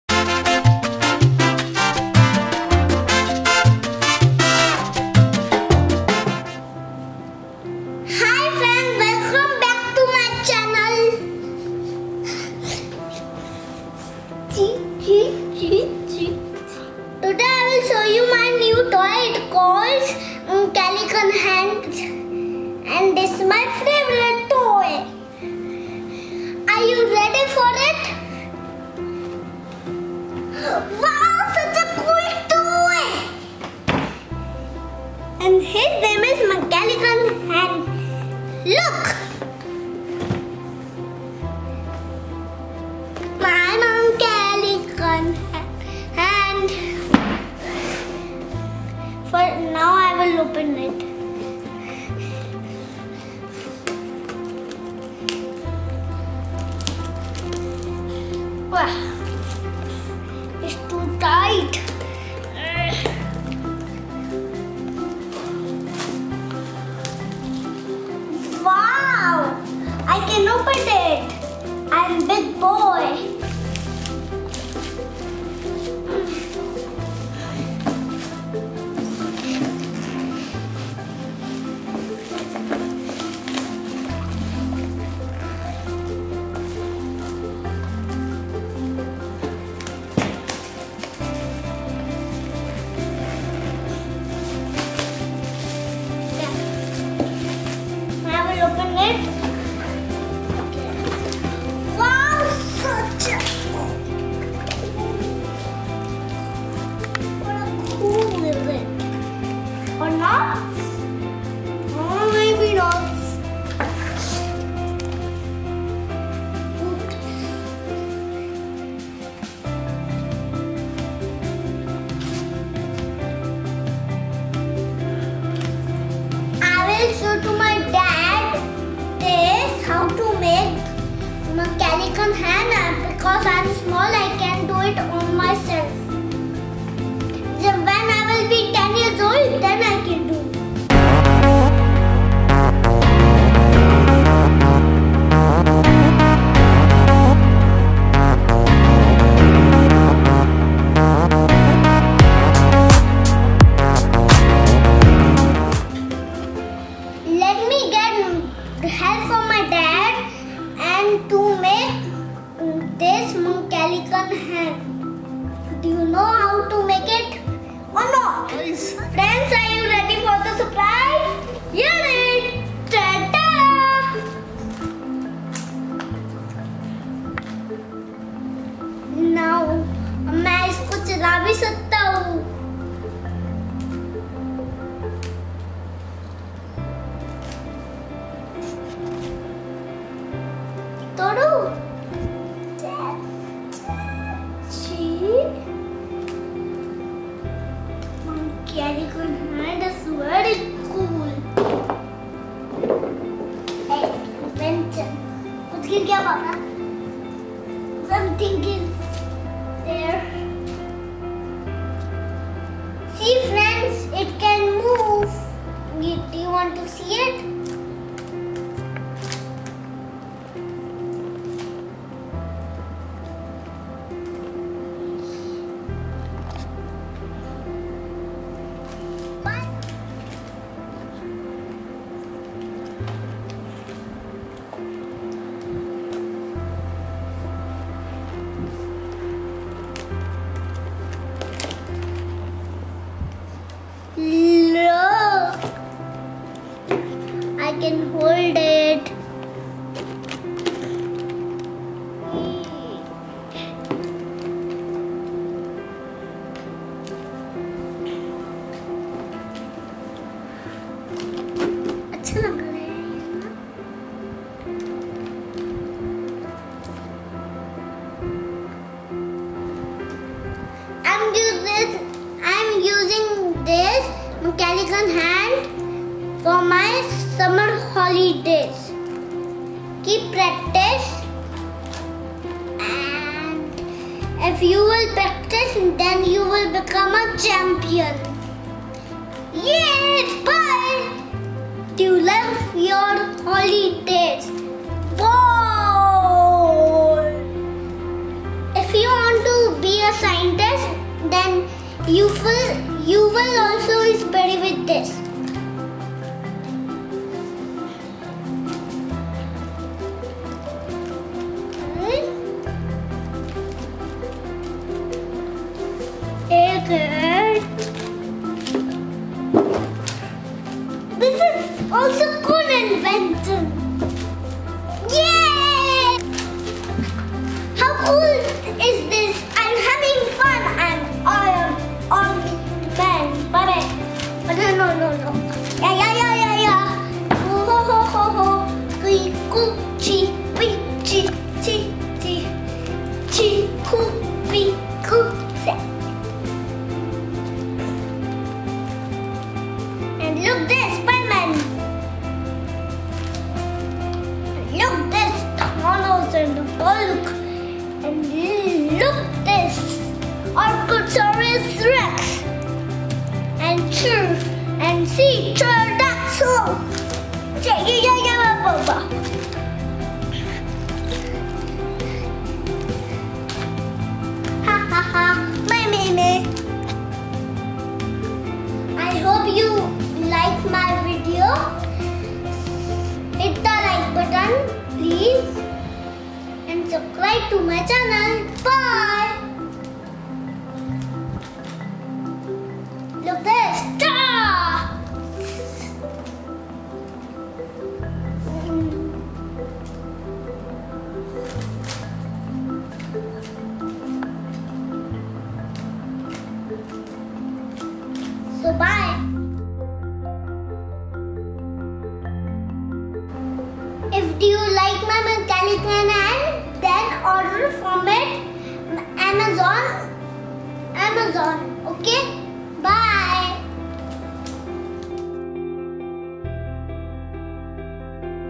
LifeOfKavir || Play with Mechanical Hand (from Smartivity). DIY. KidsLearning 3KidsEducation
learn how to make such a cool mechanical Hand and play with it.